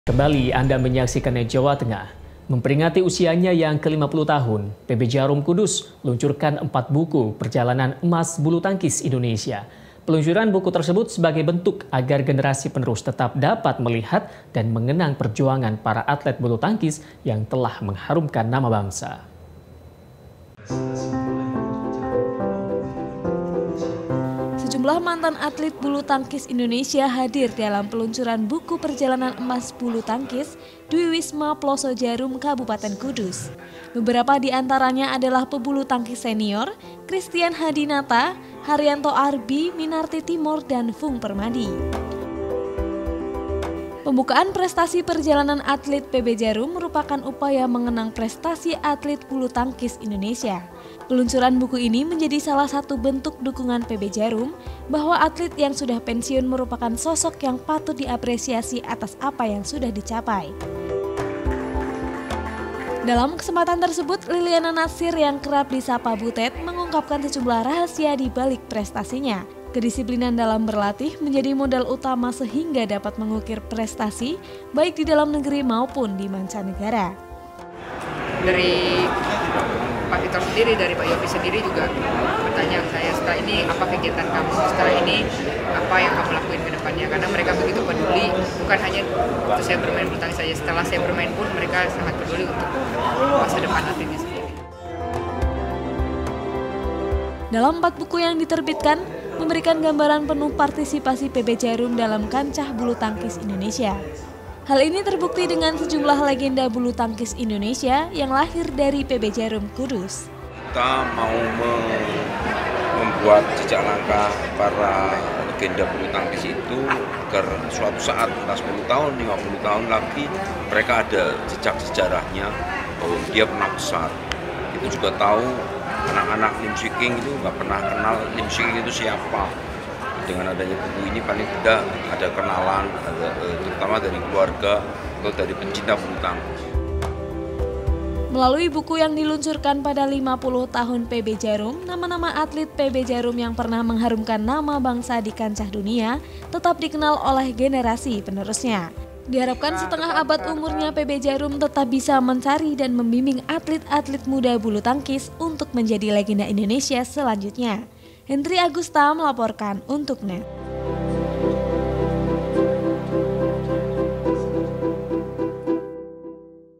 Peluncurkan Buku Perjalanan Emas Bulu Tangkis,Jadikan Ajang "reuni" Para Atletnya - NET JATENG
Sejumlah mantan atlet bulu tangkis indonesia  hadir dalam perluncuran buku perjalanan emas bulutangkis di Wisma Ploso Djarum, Kabupaten Kudus. Beberapa diantaranya adalah pebulutangkis senior, Kristian Hadinata, Haryanto Arbi, Minarti Timor dan Funk Permadi.

Pembukuan prestasi perjalanan atlet PB Djarum merupakan upaya mengenang prestasi atet bulu tangkis indonesia. Peluncuran buku ini menjadi salah satu bentuk dukungan PB Djarum, bahwa atlet yang sudah pensiun  merupakan sosok yang patut apresiasi atas apa yang dicapai.

Dalam kesempatan tersebut, Liliyana Natsir yang kerap disapa Butet,  mengungkapkan sejumlah rahasia dibalik prestasinya. Kedisplinan dalam berlatih, menjadi modal utama sehingga dapat mengukir prestasi, baik di dalam negeri maupun di mancanegara.

“Dari pak viktor sendiri pak yoppie sendiri juga bertanya ke saya setelah ini apa kegiatan kamu setelah ini apa yang kamu lakuin kedepan karena mereka begitu peduli bukan hanya waktu saya bermain bulutangkis saja tapi setelah bermain pun mereka sangat peduli untuk masa depan nantinya seperti apa.”, ujar Liliyana Natsir , Mantan Pebulutangkis.
Dalam empat buku yang diterbitkan, memberikan gambaran penuh partisipasi PB Djarum dalam kancah bulu tangkis Indonesia. Hal ini terbukti, dengan sejumlah legenda bulutangkis indonesia, yang  lahir dari PB Djarum Kudus.

"Kita mau membuat jejak langkah para legenda bulutangkis ini itu agar suatu saat entah sepuluh atau 50 tahun lagi mereka ada jejak sejarahnya bahwa dia pernah besar, kita juga tau anak anak lim swiking ga pernah kenal lim swiking itu siapa dengan adanya buku ini paling tidak ada kenalan terutama dari keluarga atau dari pecinta bulutangkis", ujar Yoppy Rosimin, Program Director Bakti Olahraga Djarum Foundation

Melalui buku yang di luncurkan pada lima puluh tahun PB Djarum, nama-nama atlet PB Djarum yang pernah mengharumkan nama bangsa di kancah dunia, tetap dikenal oleh generasi penerusnya. Diharapkan, setengah abad umurnya PB Djarum tetap bisa terus mencari dan membimbing atlet atlet muda bulutangkis, untuk menjadi legenda Indonesia selanjutnya.